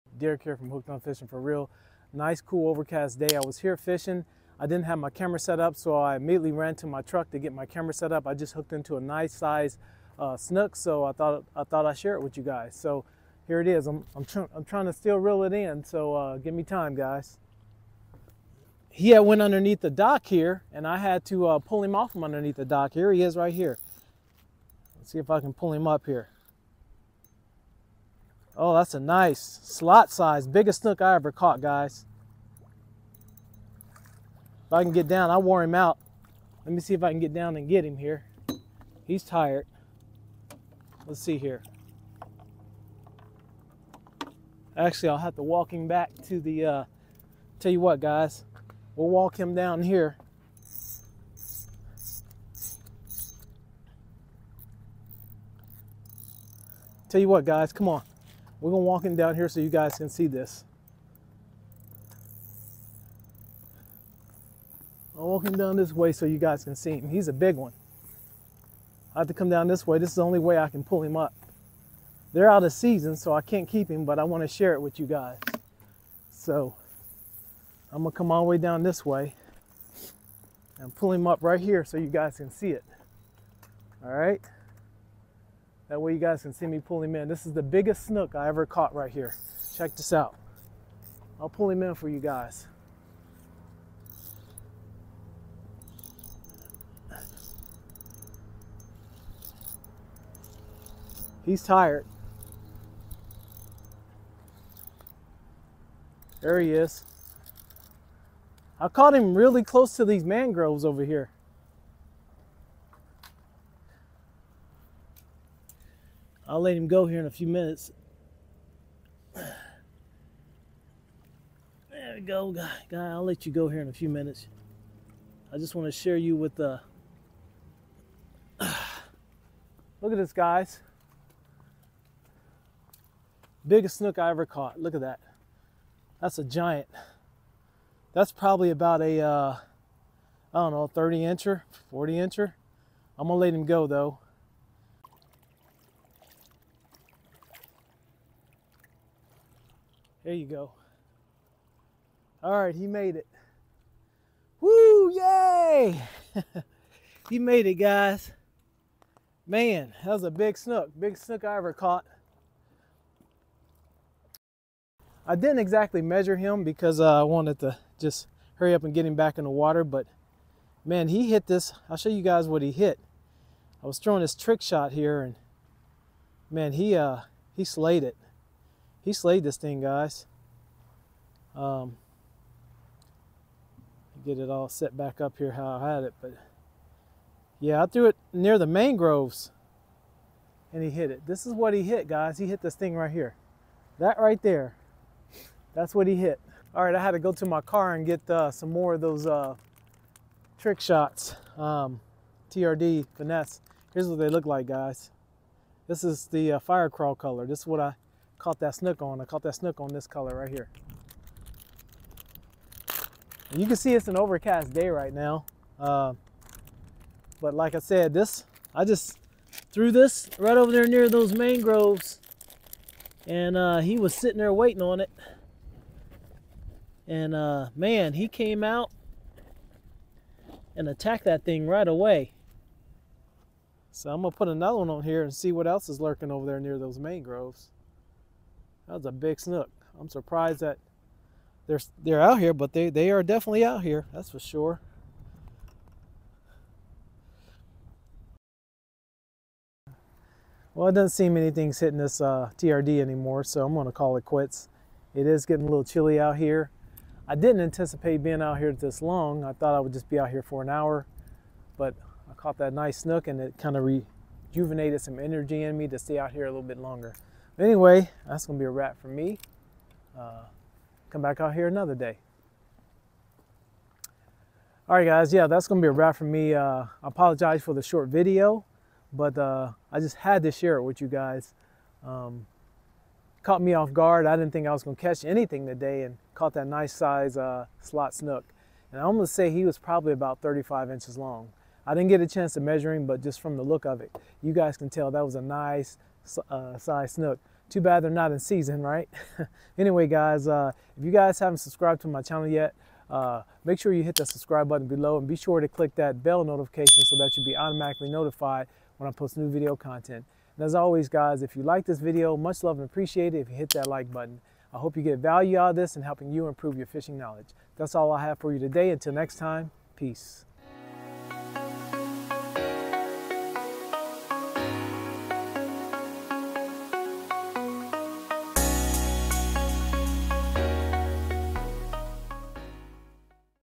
Fishing From Dock And Got Surprised.
Decided to fish from a Dock and got surprised with an unexpected catch.  Never caught a Slot snook before.  This is by far the biggest Snook I ever caught.  Hope you all  enjoy the video as much as I enjoyed sharing this special catch with you.